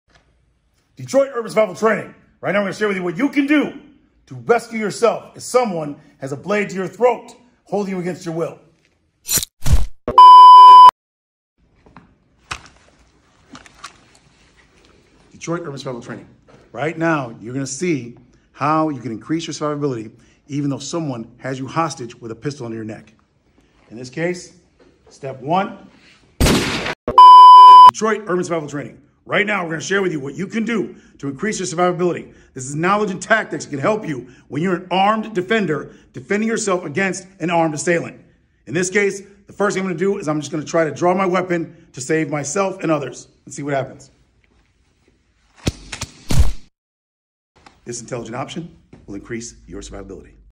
How NOT To Survive...
Today we cover the BEST Survival tactics around, presented by Detroit Urban Survival Training.